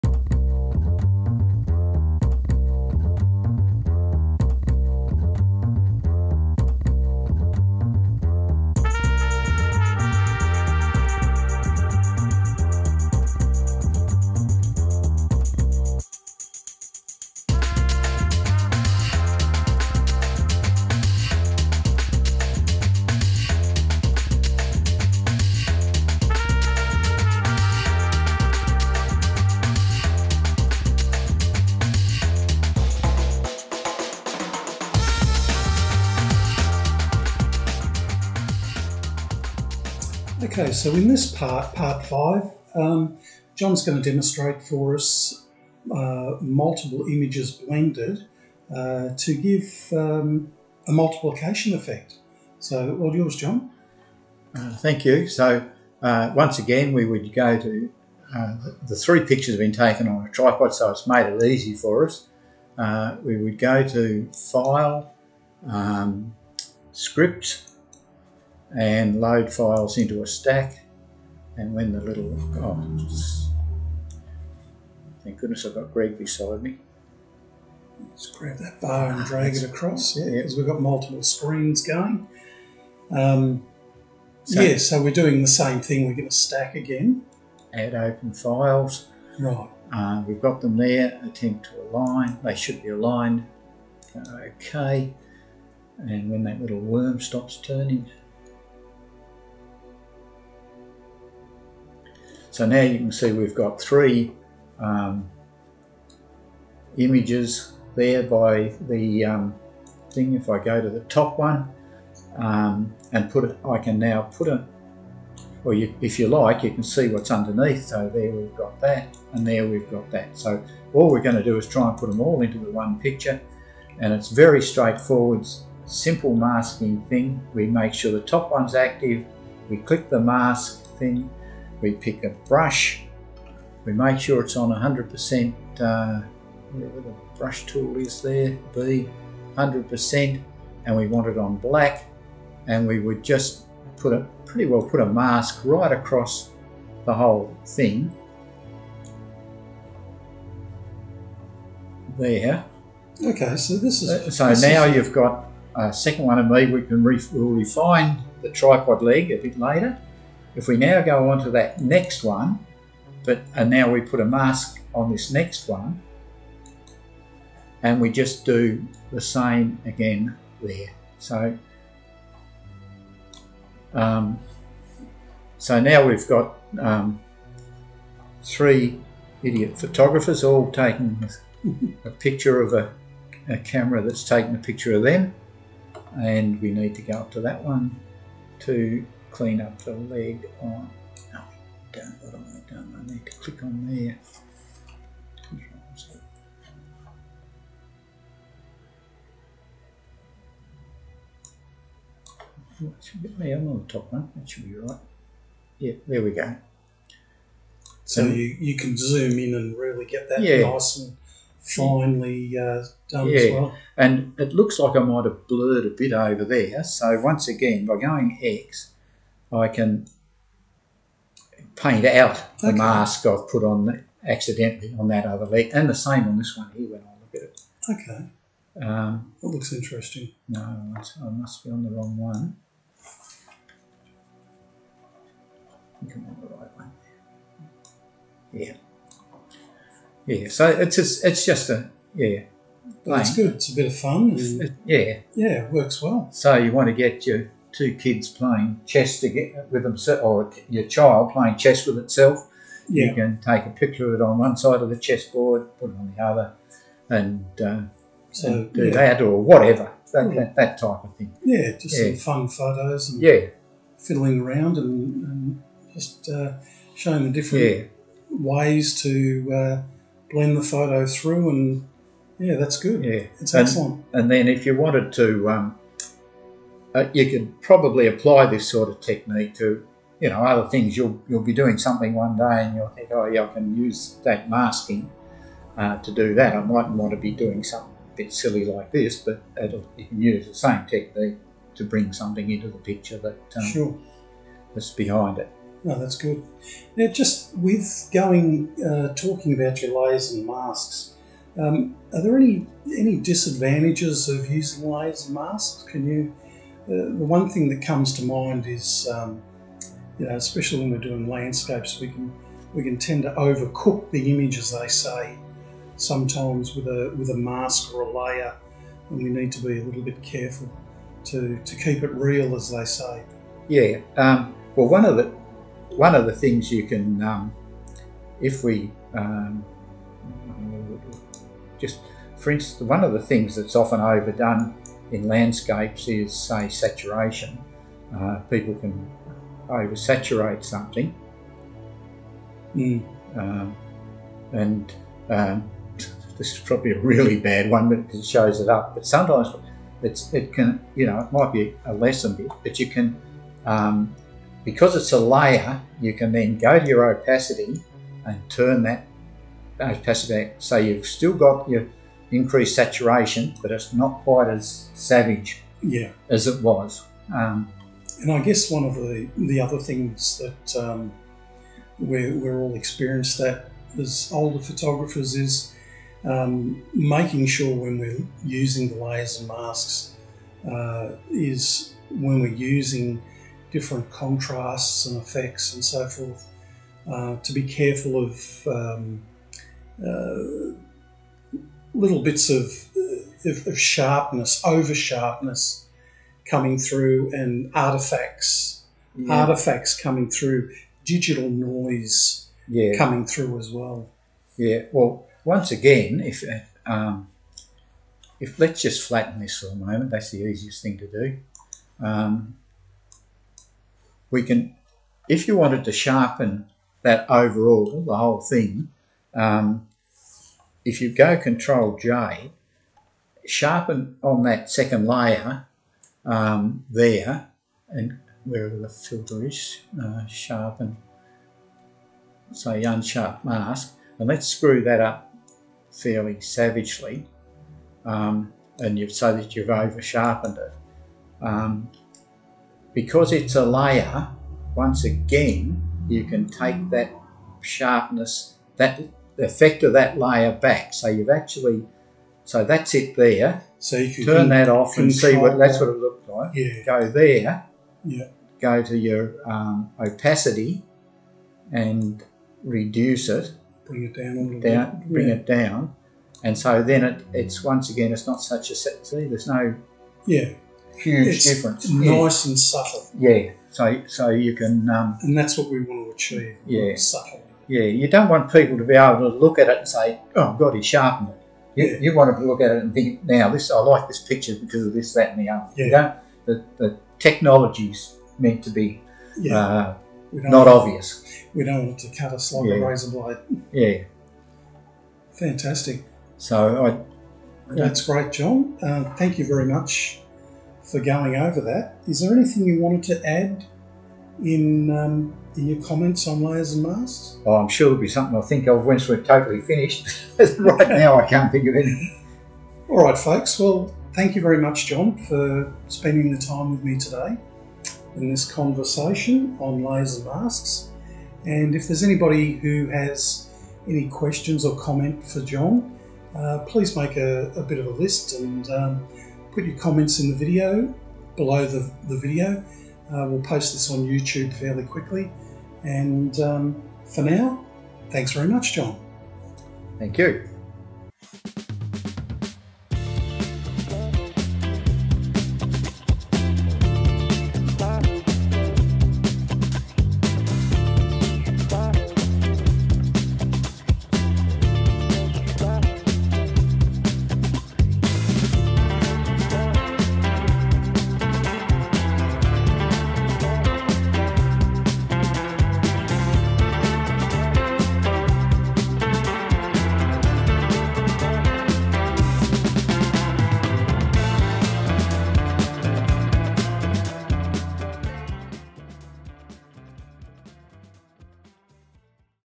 Stawell Camera Club - Layers & Masks Pt 5 - John Tiddy
This is Part 5. (final video) of our 5 part series on "Layers & Masks" for the members of Stawell Camera Club. These videos will cover the basic concepts of using Layers & Mask in Adobe Photoshop that John Tiddy will explain.